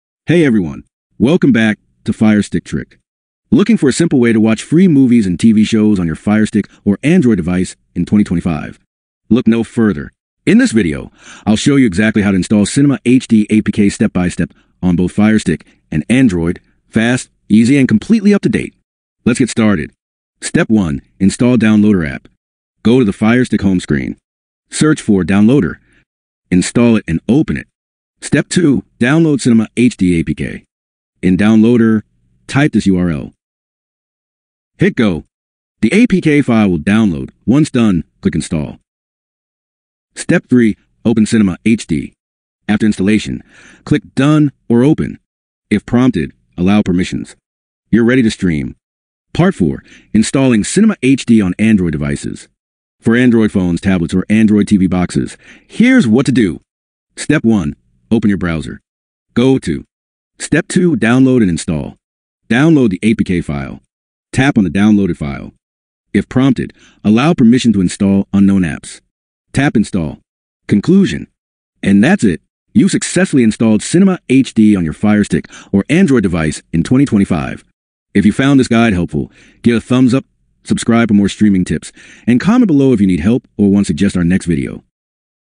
Install Cinema HD APK on Firestick/Android (2025 Update)!
Looking to stream your favorite movies and TV shows for free? In this updated 2025 guide, I’ll show you how to install Cinema HD APK on Firestick, Fire TV, and Android devices in just a few easy steps.

Cinema HD is one of the most reliable streaming apps that gives you access to a huge collection of movies and shows in HD quality. Whether you're using an Amazon Firestick or any Android phone or tablet, this tutorial covers it all!

✅ What You'll Learn in This Video:
How to enable apps from unknown sources,
How to download and install Downloader on Firestick,
How to install Cinema HD APK safely,
Best settings for smooth streaming,
Tips for keeping the app updated in 2025,

📥 Working Cinema HD APK URL (2025)
(Updated in video – watch till the end!)

📺 Whether you're a beginner or experienced user, this guide is designed to help you get started quickly and easily.